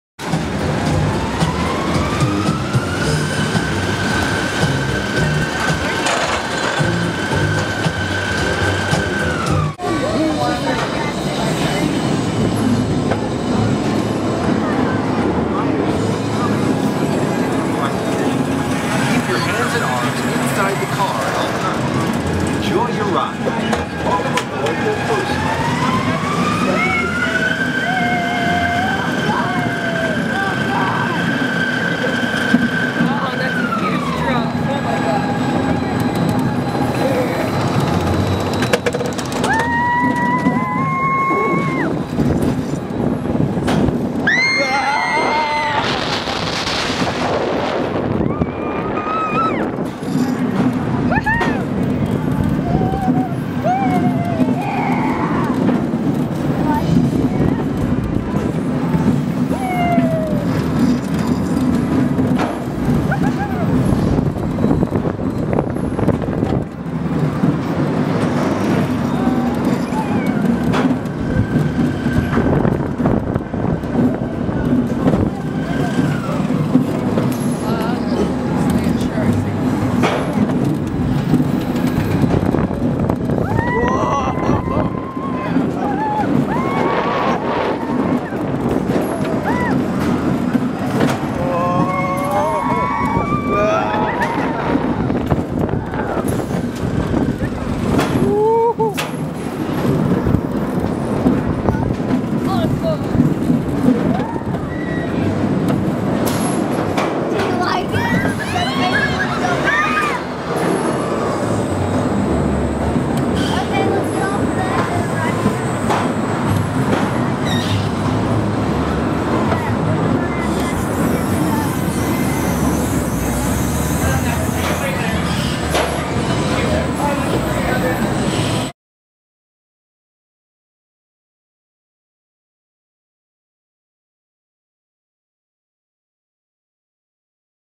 Coast Rider - Daytime (HD POV) - Knott's Berry Farm
Enjoy this Daytime HD POV of Knott's brand new ride, Coast Rider! Coast Rider is one of three new attractions at Knotts Berry Farms Boardwalk. Come over this summer and check them out.

Keep Calm and ride On!
-Theme Park Dan